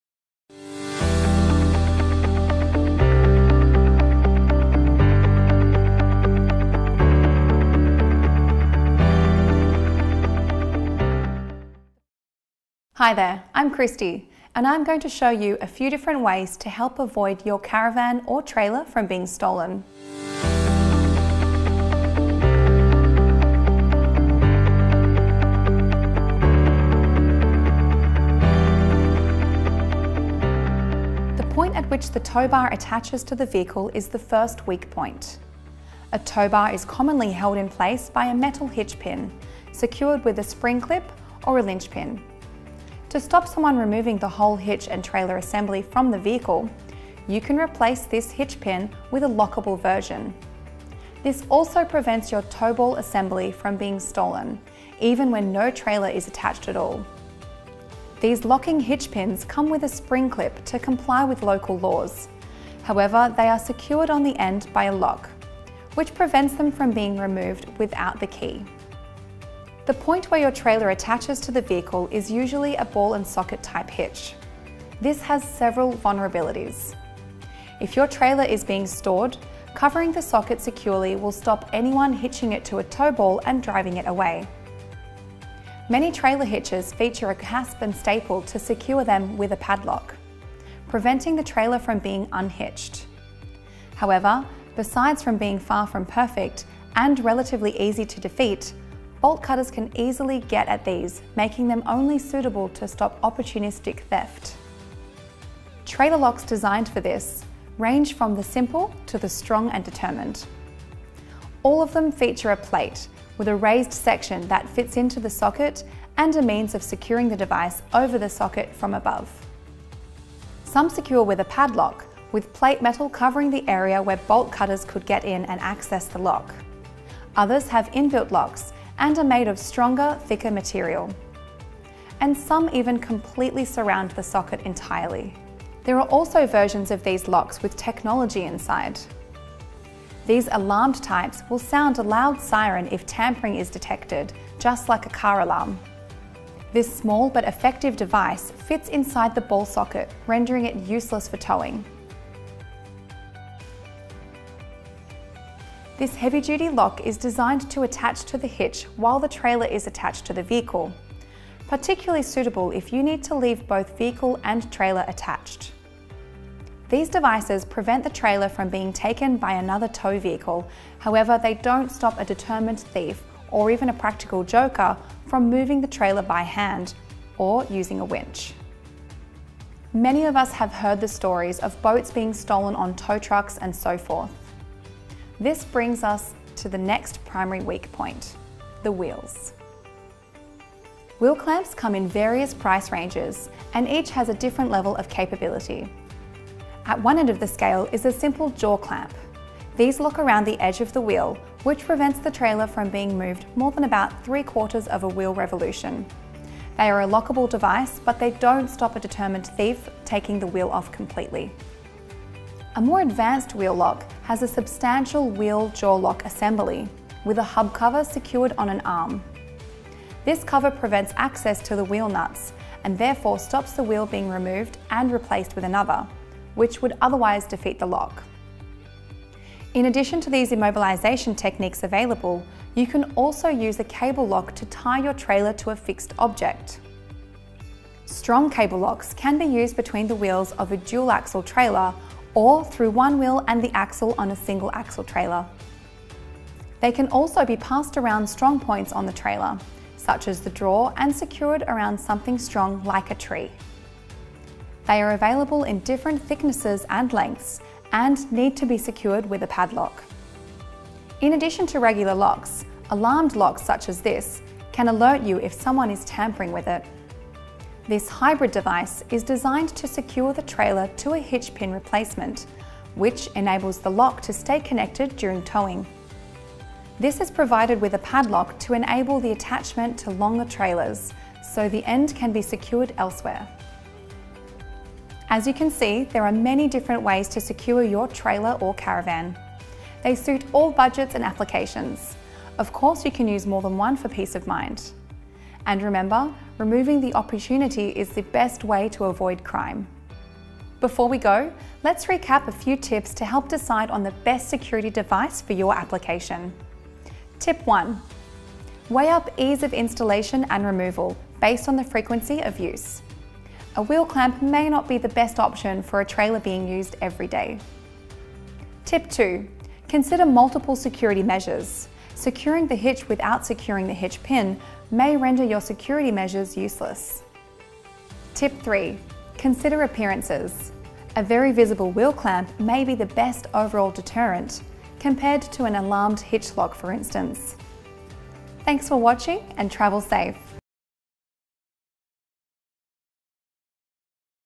RTM Presents Trailer Security
Join Kristy as she guides you through our range of trailer security products. This range review covers your tow hitch, wheel locks right through to alarmed goodies for both parked or attached trailers and caravans. This is perfect for peace of mind and the security of your investment.